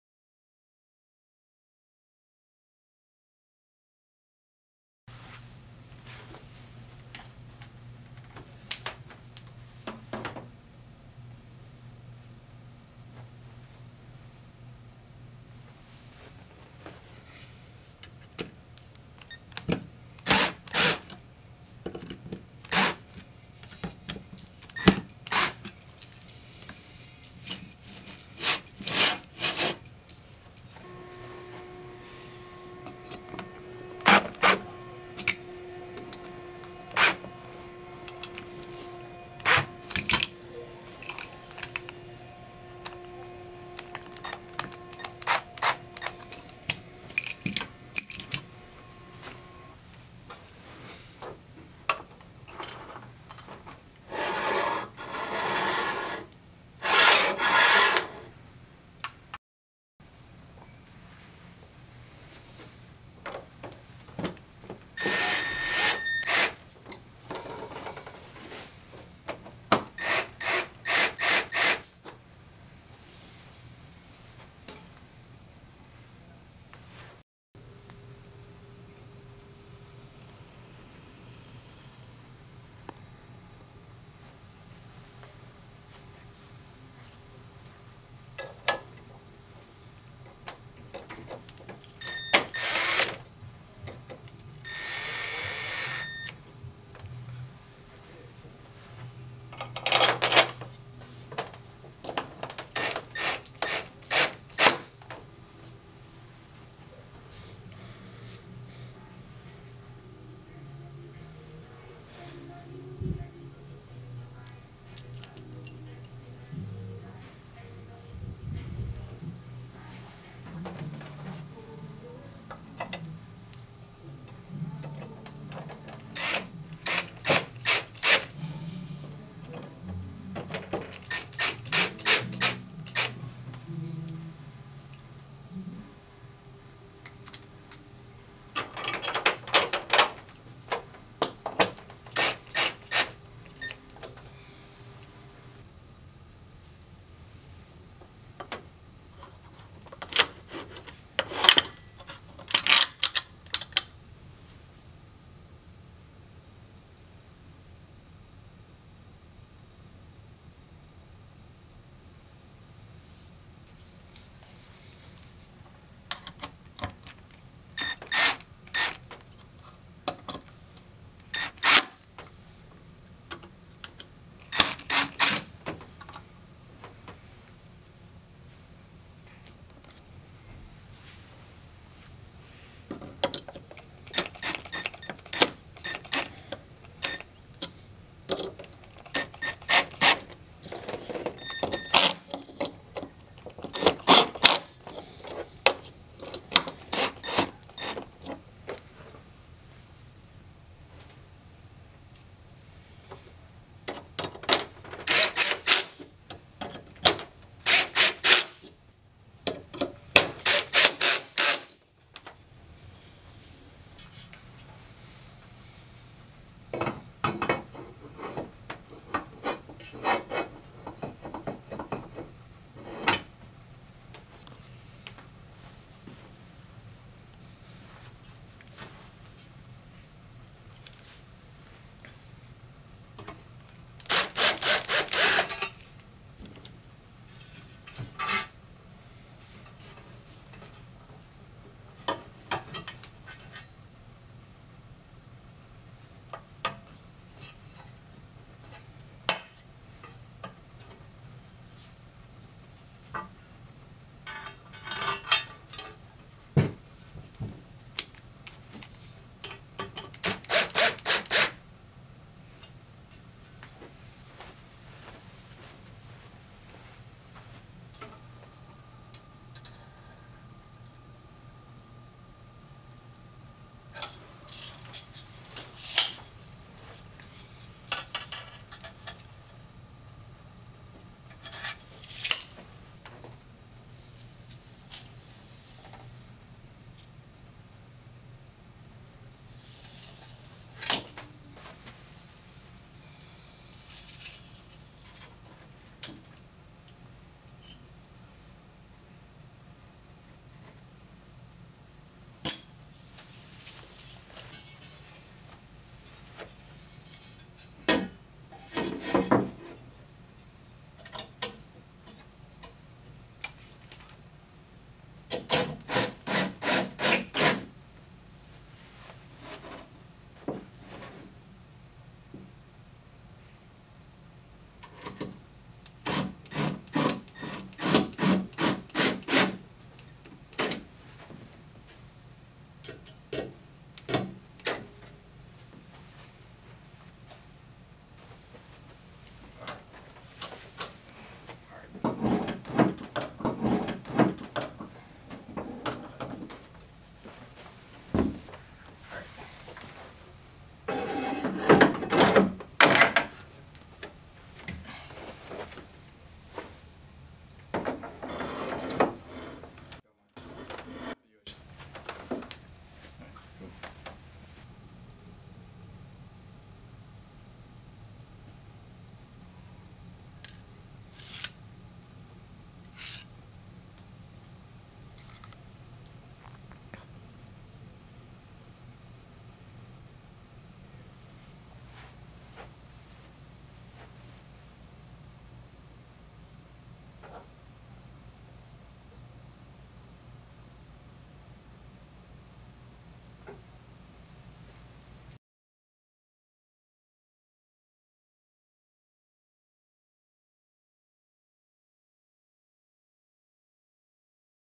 James Loudspeaker | SoundBar Mounting Bracket Installation: BRK.TV.UN
Installation video for use with a James Loudspeaker BRK.TV.UN universal mounting bracket